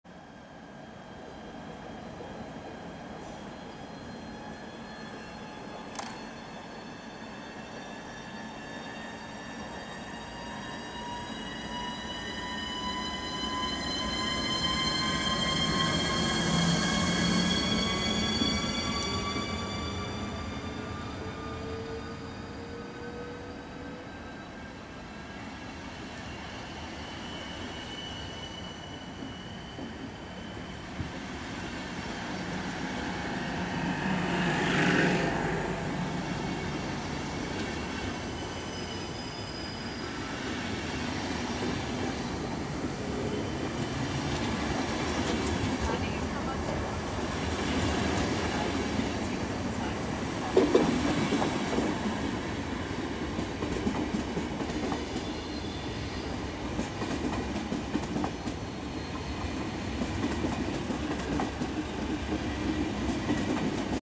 EuroCity 104 Sobieski Wien Hbf
EC 104 Wien Hbf - Gdynia Glowna. Poslední dva vozy: Wien Hbf - Krakow Glowny. Video pořízeno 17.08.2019.